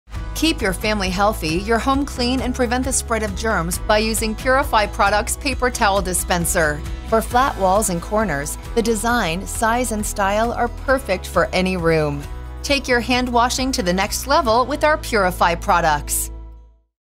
Purify Products - Uniquely Designed Paper Towel dispensers and Accessories (15-sec)
The Coronavirus Pandemic has led us to make Better Hygiene the New Normal. The nation has a collective heightened awareness of good hygiene. We should all take the necessary steps to ensure our family, friends, co-workers, and others have access to a clean and sanitary environment.

Purify products provide protection against unseen microbes that can cause serious harm.
As we shift into this new normal, we hope Purify Products are your go-to company for products and solutions that will improve and enhance your everyday living.